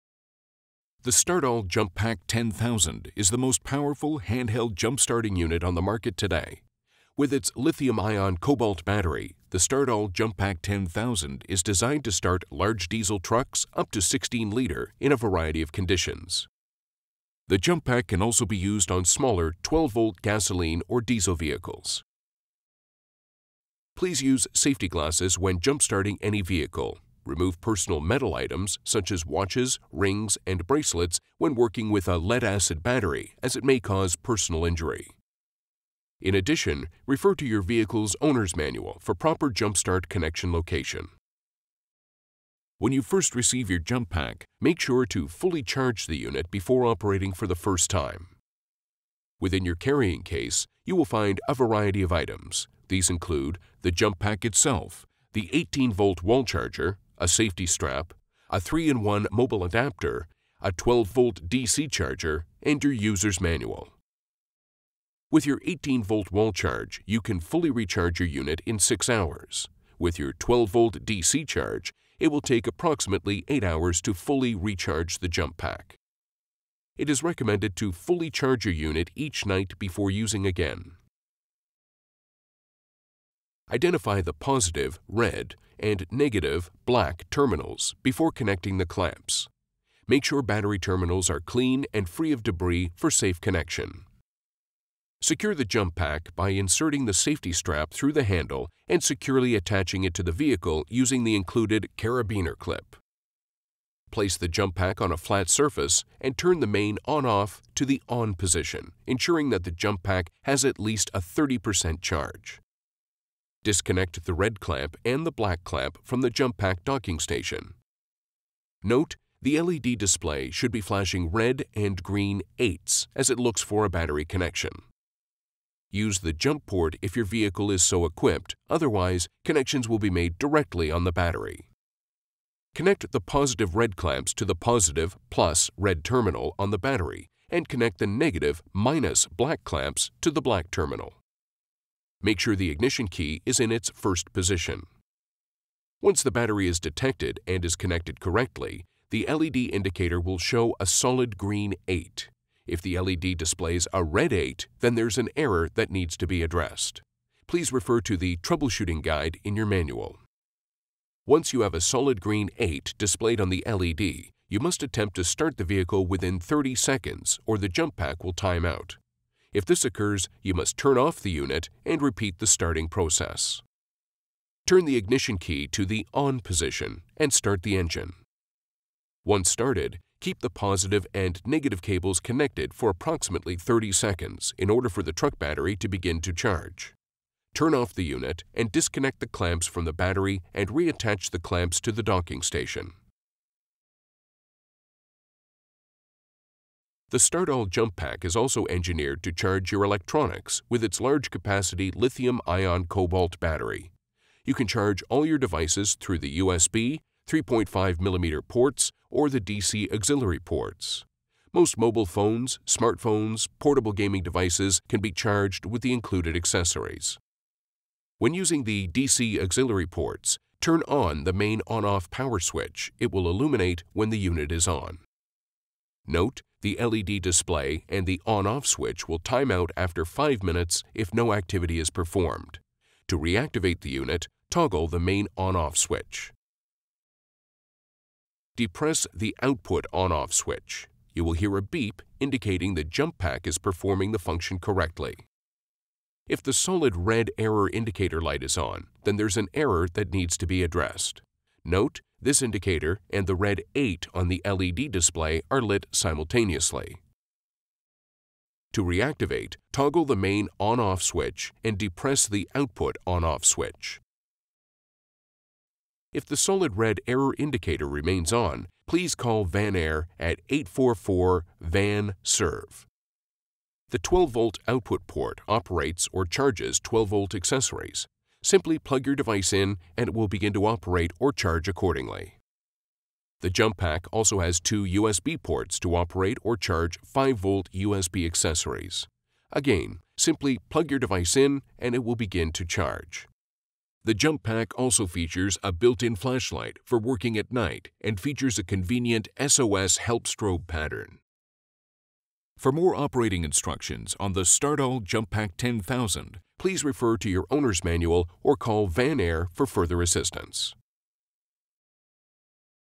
Start-All® Jump-Pack 10,000A Operating Procedures
Watch and learn how to use the Start-All® Jump-Pack 10,000 Series. Built to last, the 10,000A, 1332005S joules, 12v Start-All® Jump-Pack starts the toughest Class 8 engines on demand.